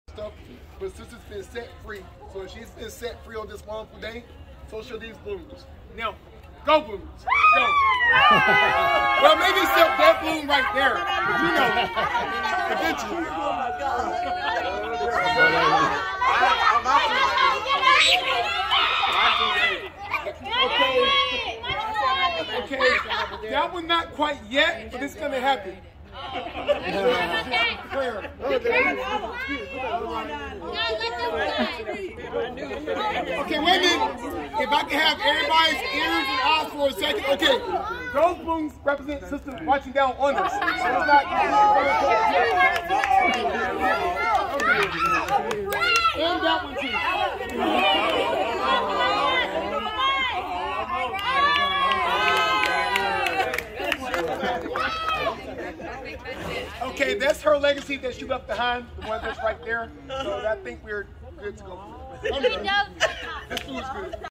Sister's Legacy
Balloons released in memory of Myrdice (Sister) Menefee